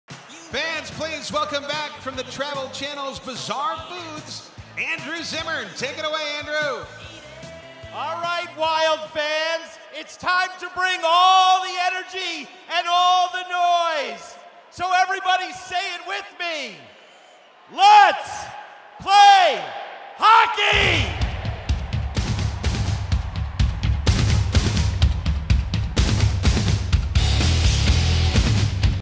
Let's Play Hockey!
I had the honor of kicking off a Minnesota Wild game on April 11, 2013. It was a blast. Go Wild!